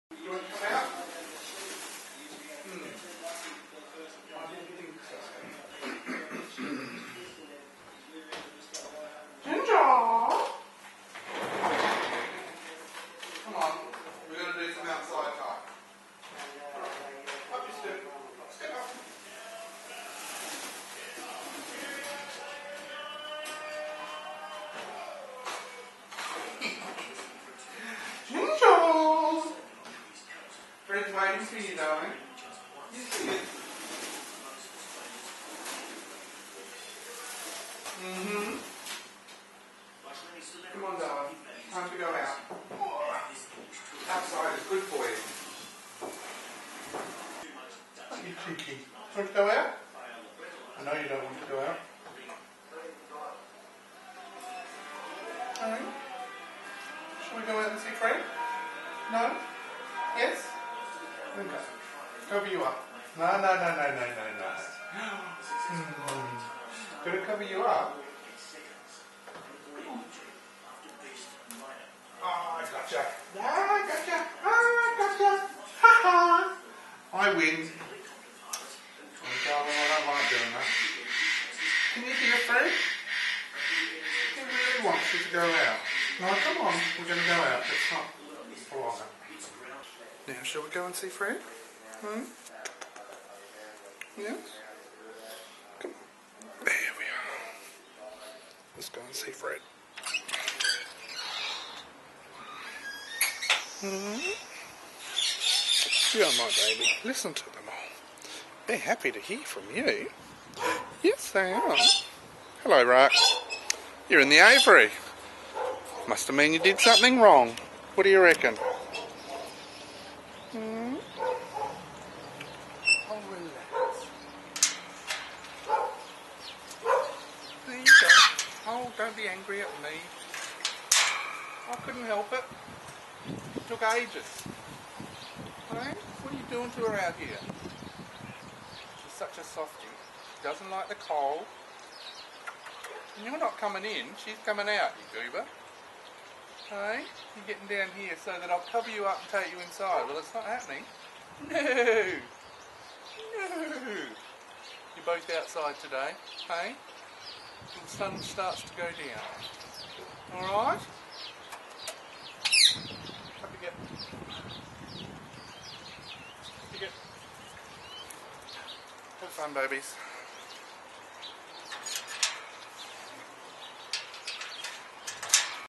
Teasing dad
Ginger just refuses to enjoy the great outdoors.